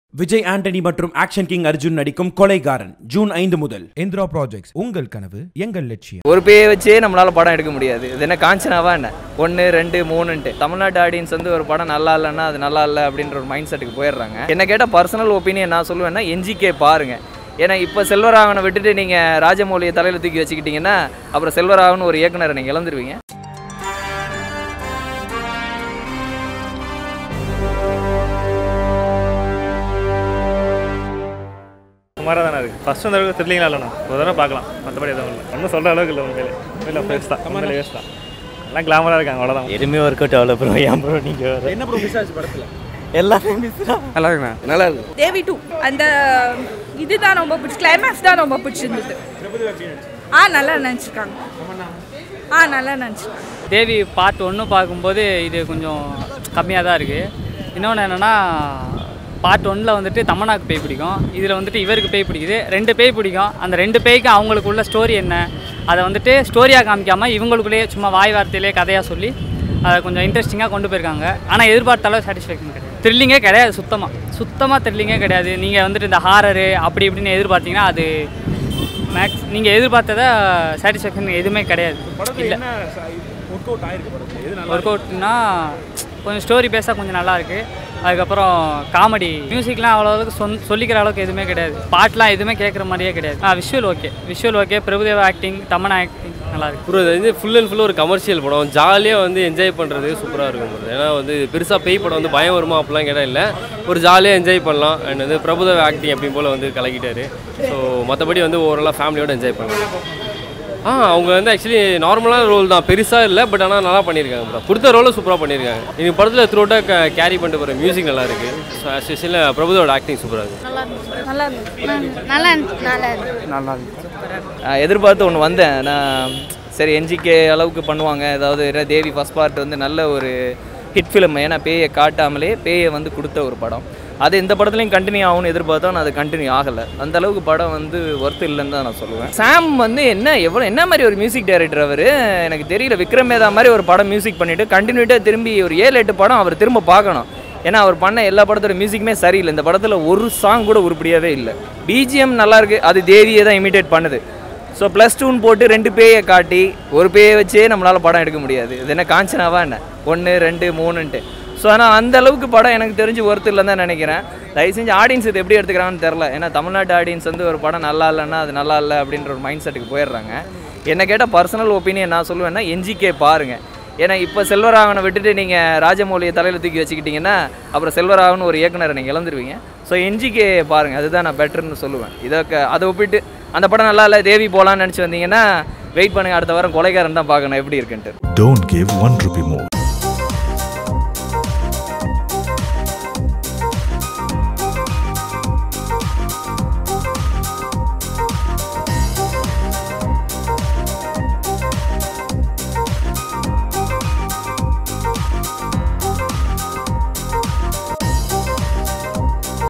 Devi 2 FDFS Public Review | Prabhu Deva | Tamannaah | Nandita Swetha | A. L. Vijay
Public Review of Devi 2  From Prabhu Deva and A. L. Vijay combo